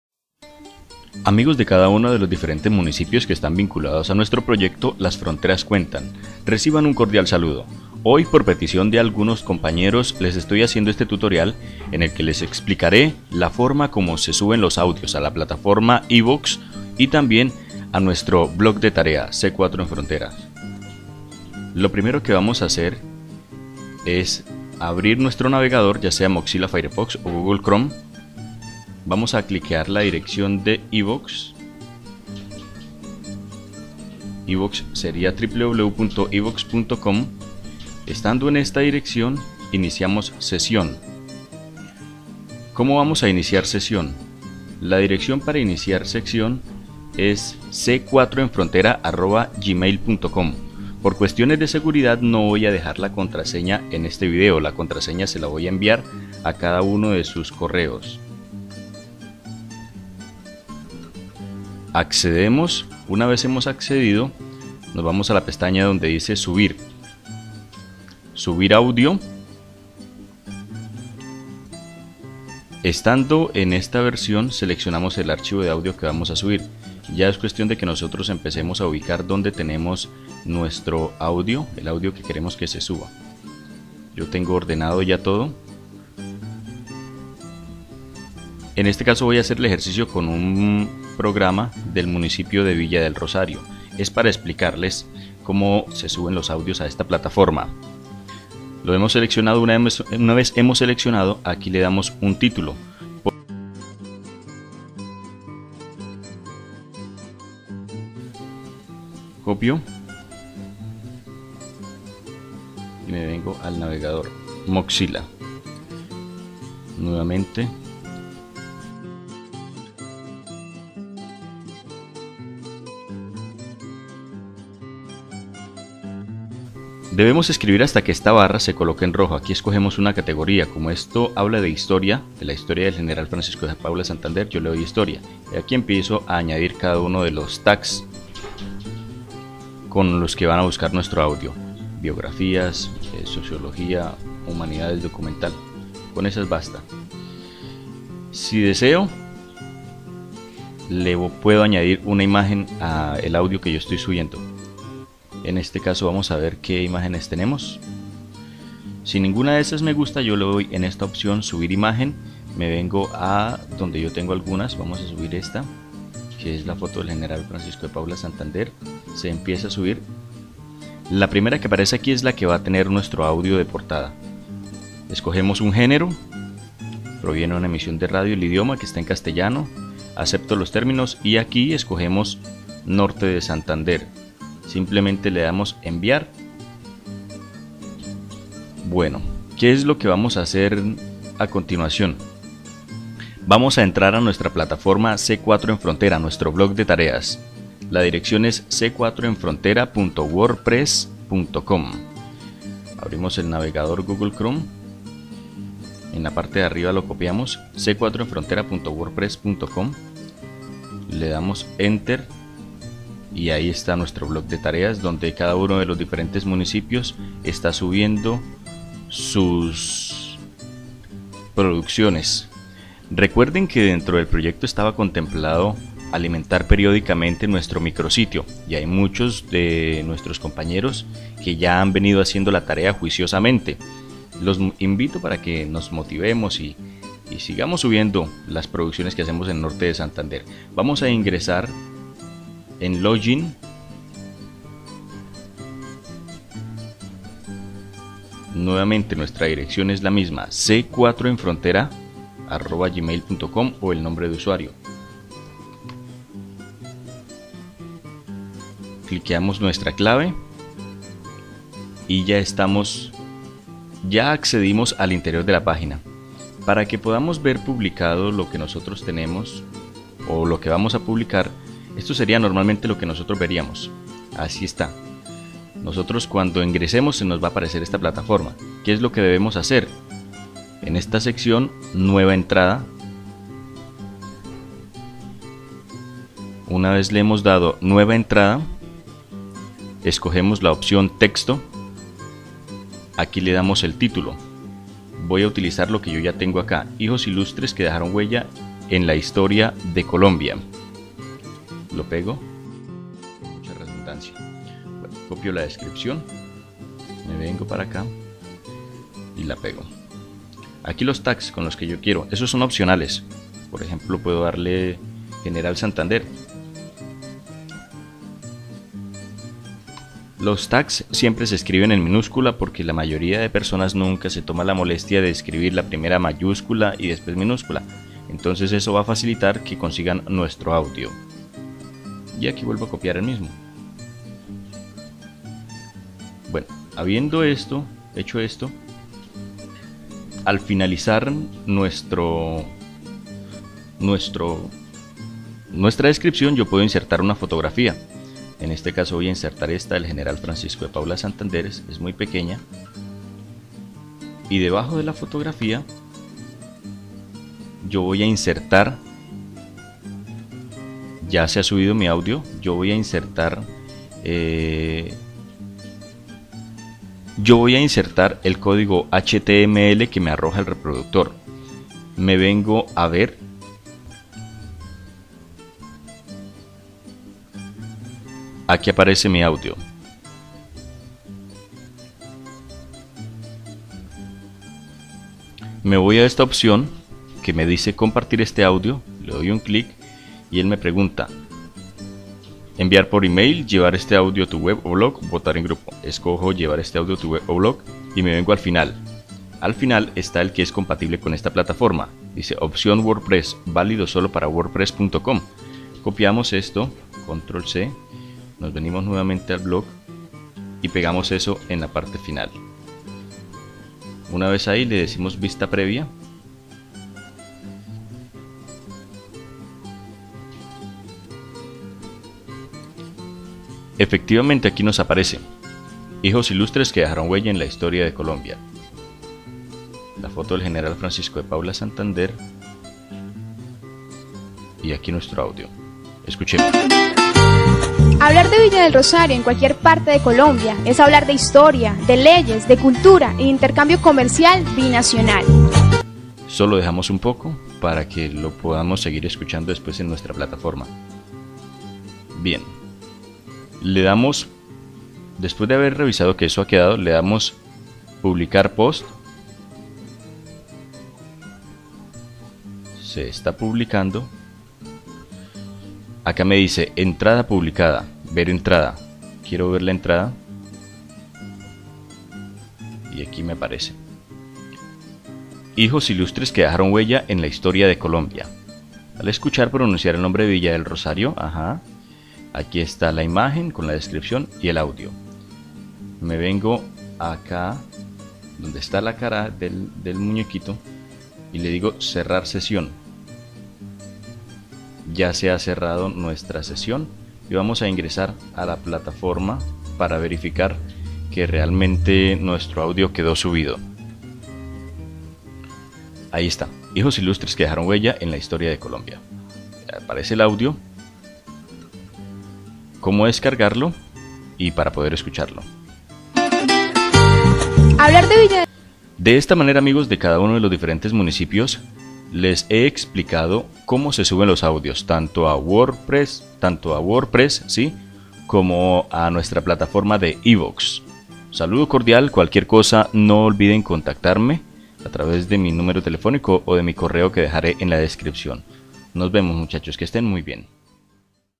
subir archivos a ivoox y word press
En este tutorial se explica como se suben los audios a el canal podcast ivoox para luego ser publicados en la plataforma word press